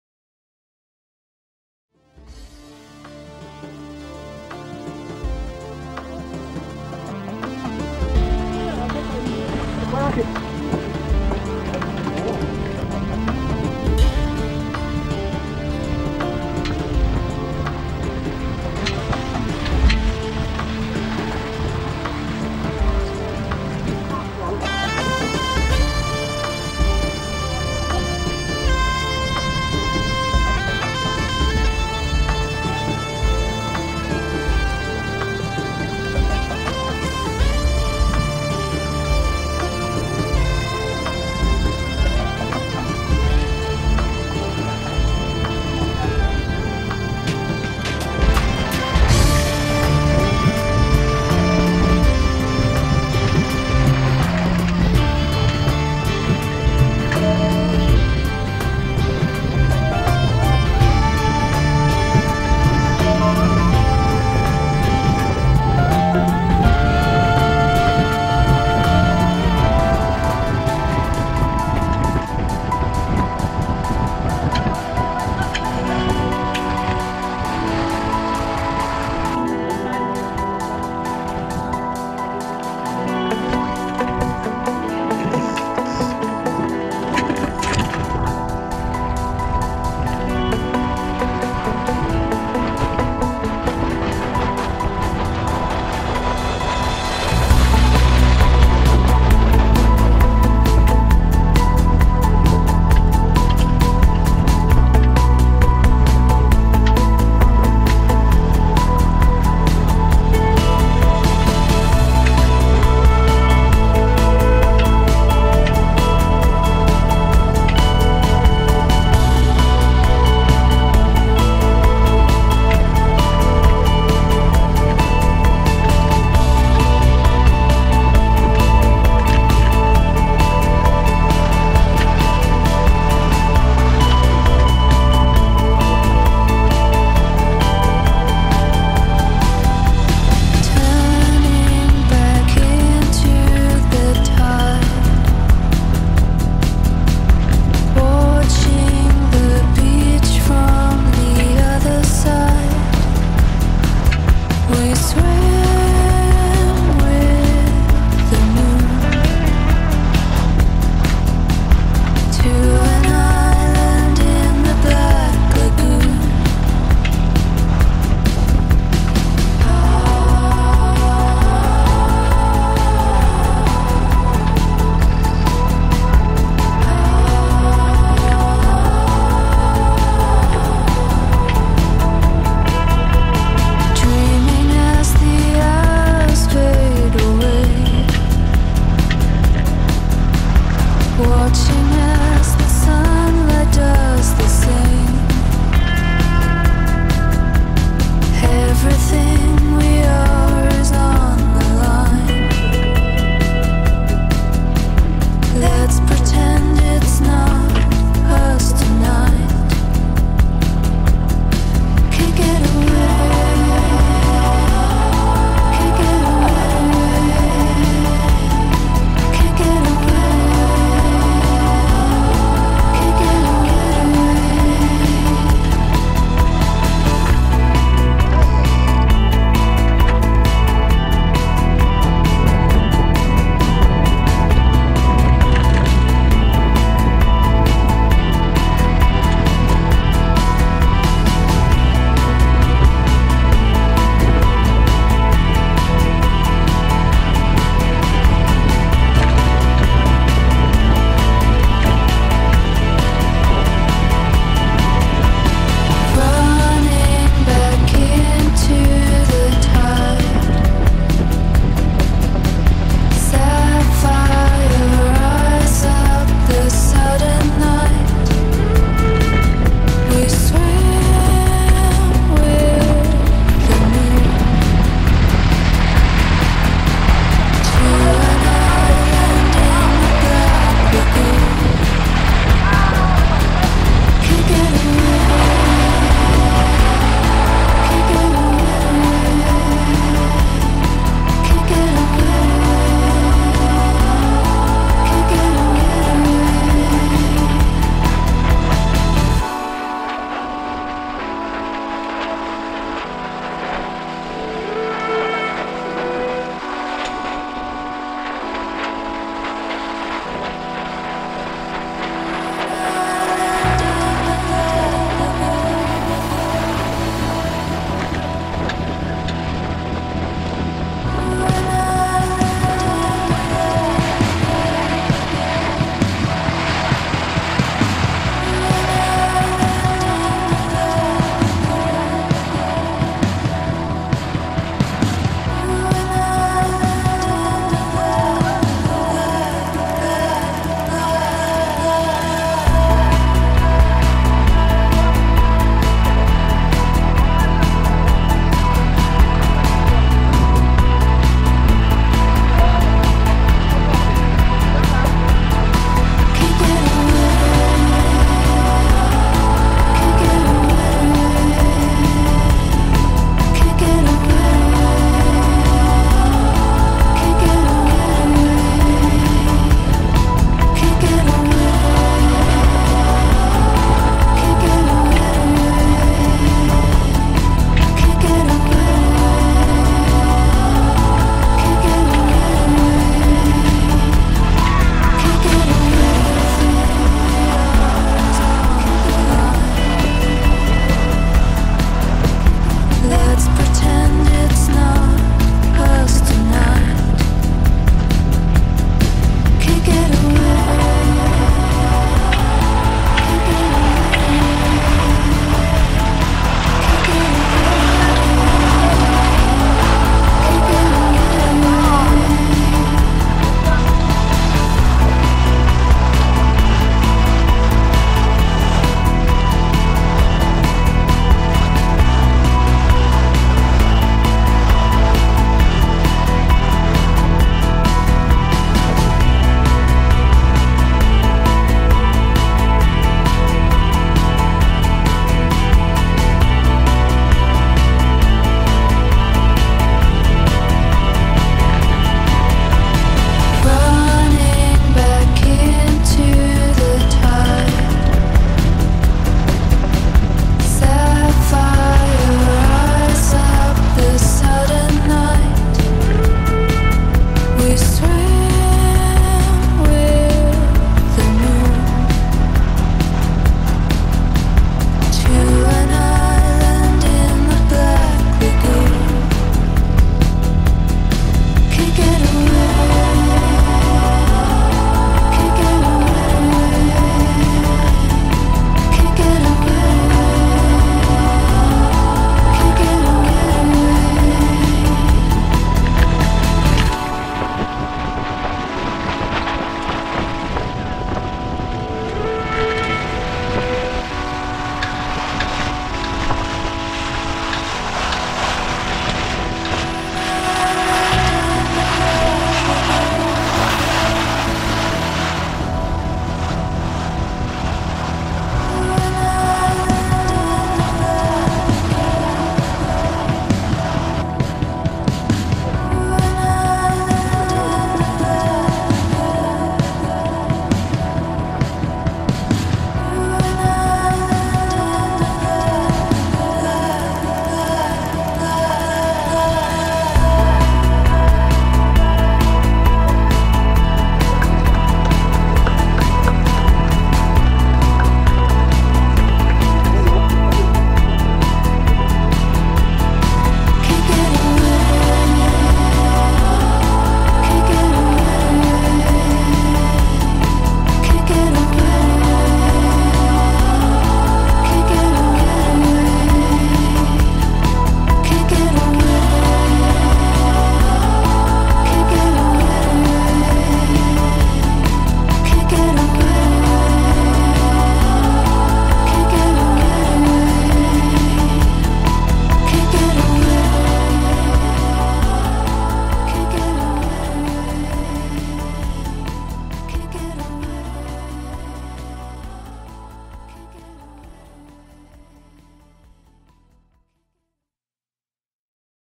BBB Chasseurs de risées
Chasseurs de risées
un bel après midi d'orage et soleil, à traquer la risée et s'envoler
Pauline, Charles et PP